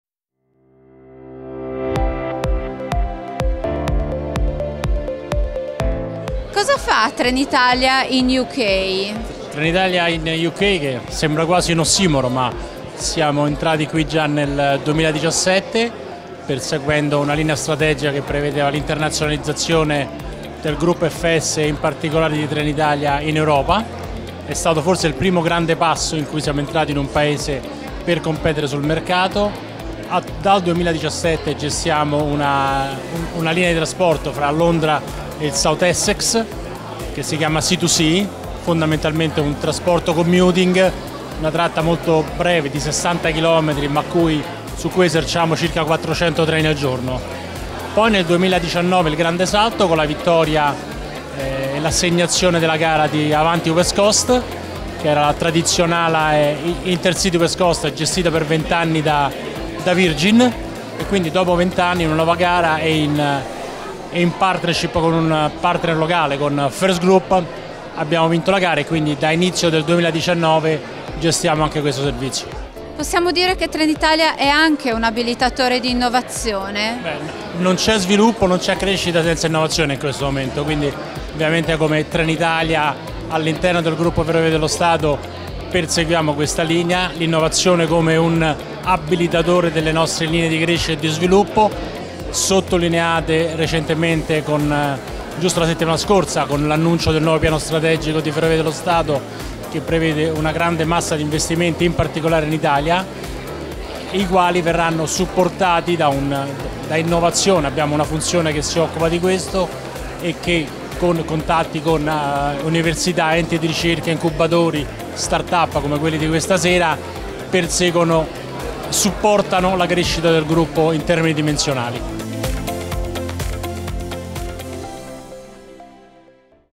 Smau Londra 2022 | Intervista a Ernesto Sicilia, Trenitalia UK
A Smau Londra abbiamo intervistato Ernesto Sicilia, International Business Director Trenitalia and Managing Director Trenitalia UK: “Trenitalia in UK  sembra quasi un ossimoro, ma siamo entrati qui già nel 2017 perseguendo una linea strategica che prevedeva l'internazionalizzazione del gruppo FS, e in particolare di Trenitalia, in Europa.  E’ stato forse il primo grande passo in cui siamo entrati in un paese per competere sul mercato.  Dal 2017 gestiamo una linea di trasporto fra Londra e South Essex, fondamentalmente un trasporto commuting, una tratta molto breve di 60 km su cui gestiamo circa 430 treni al giorno.  Nel 2019 il grande salto con la vittoria e l’assegnazione della gara Avanti West Coast gestita  per 20 anni da Virgin (...). Quindi da inizio del 2019 gestiamo anche questo servizio.  Non c’è crescita nè sviluppo senza innovazione. Come Trenitalia all’interno del gruppo Ferrovie dello Stato perseguiamo questa linea: innovazione come abilitatore delle nostre linee di crescita e di sviluppo ancora più sottolineate nel nuovo piano di sviluppo di Ferrovie dello Stato che prevede una grande massa di investimenti in particolare in Italia, che verranno supportati dalla nostra funzione Innovazione che grazie a contatti con università, centri di ricerca, start up, sostengono il gruppo nella crescita”.